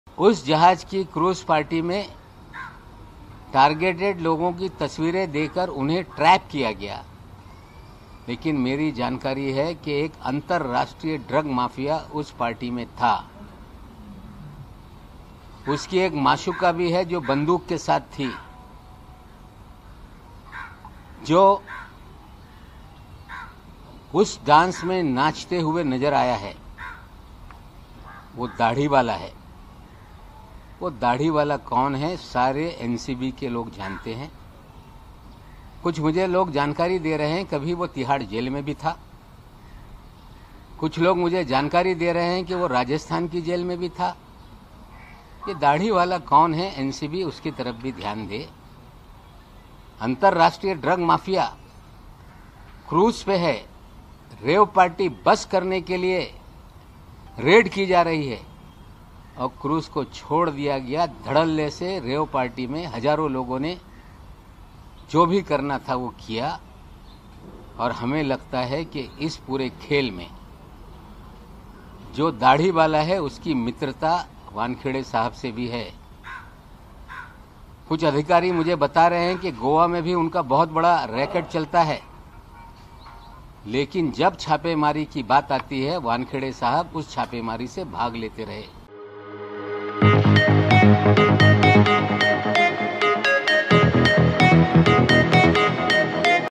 ‘ପାର୍ଟିରେ ଥିବା ଦାଢ଼ୀ ବାଲା କିଏ’ | Odisha Reporter
ଏକ ଆନ୍ତର୍ଜାତୀୟ ଡ୍ରଗ୍ ମାଫିଆ, ତାଙ୍କ ଗର୍ଲଫ୍ରେଣ୍ଡ ଡ୍ରଗ୍ ପାର୍ଟୀରେ ଥିଲେ | ତାଙ୍କର ଦାଢ଼ି ଅଛି।  ଏନସିବିରୁ ସମସ୍ତେ ଜାଣନ୍ତି ସେହି ଦାଢ଼ି ଥିବା ବ୍ୟକ୍ତି କିଏ। ଏନସିବି ମଧ୍ୟ ତାଙ୍କୁ ଖୋଜିବା ଉଚିତ୍ | ଆମେ ଅନୁଭବ କରୁଛୁ ଯେ ସେ ସମୀର ୱାନକେଡେଙ୍କ ବନ୍ଧୁ ବୋଲି କହିଛନ୍ତି   ମହାରାଷ୍ଟ୍ର ମିନ ନବାବ ମଲ୍ଲିକ |